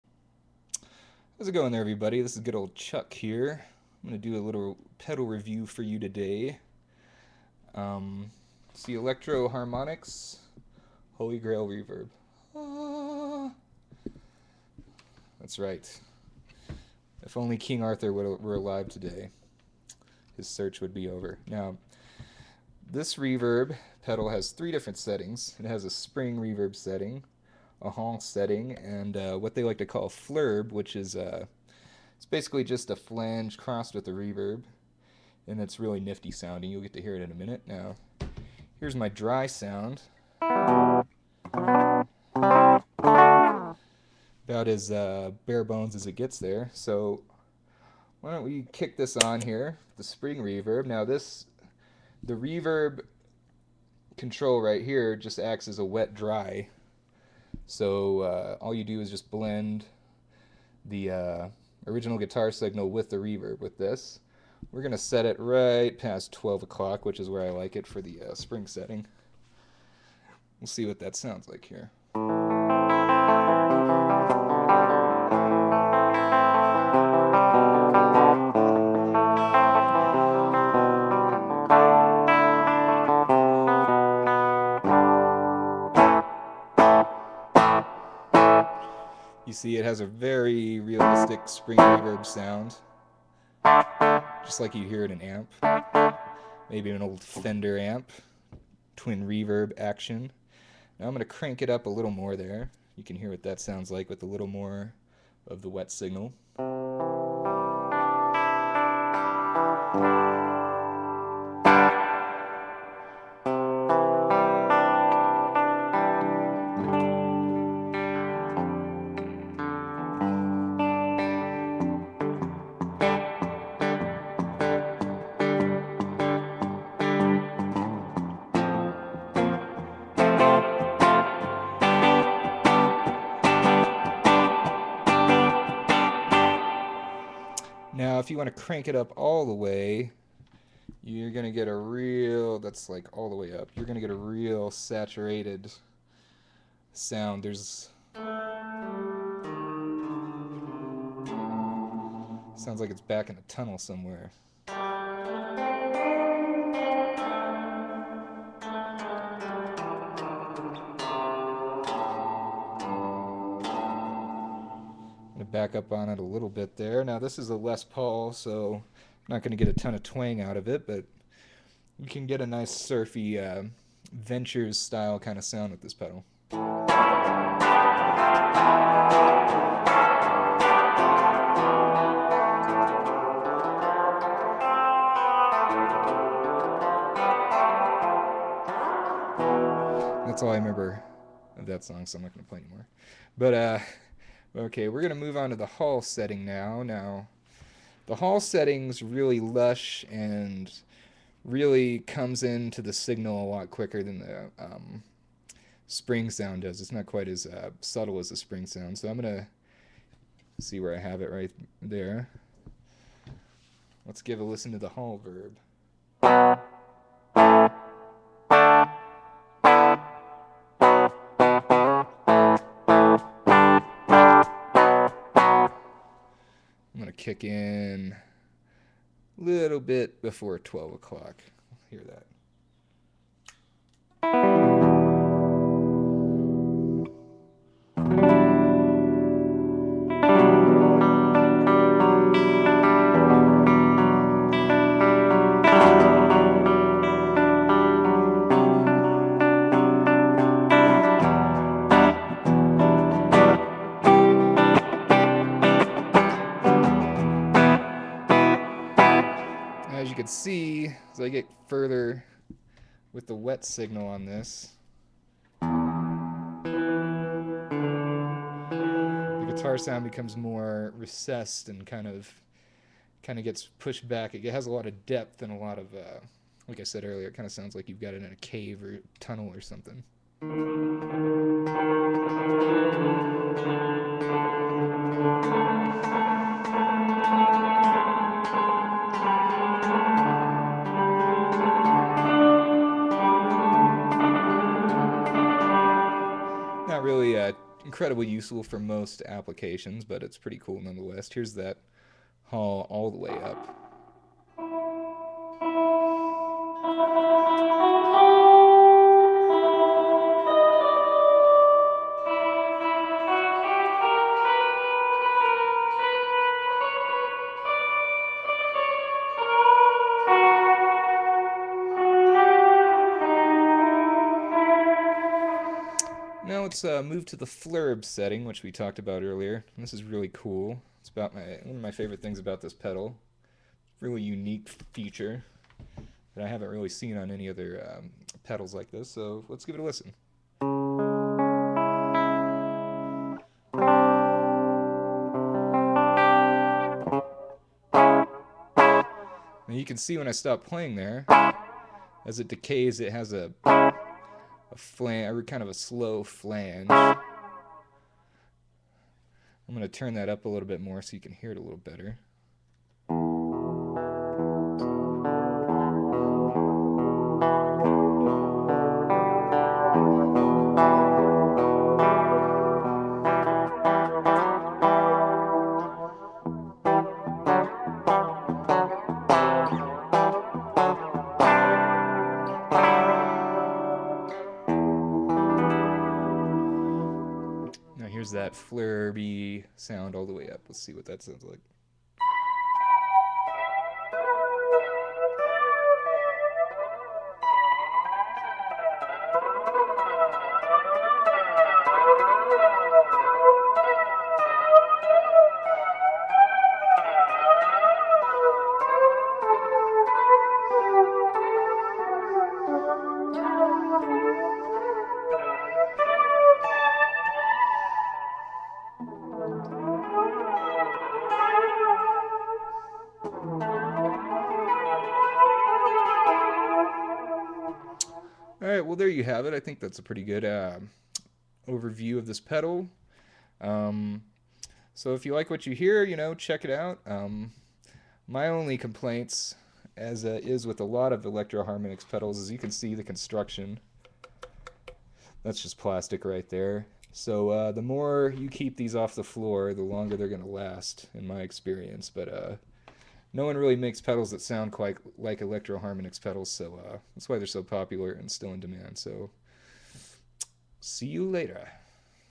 Electro-Harmonix Holy Grail Reverb Review
This is a great reverb pedal, used by John Frusciante, Nels Cline, Pepper Keenan, etc. NOTE: Do not adjust your set this video freezes occasionally there was a problem during the recording but it comes back after a few secs. And check out the Dave Weiner review of the Holy Grail+; he's actually a ridiculously better guitar player than me.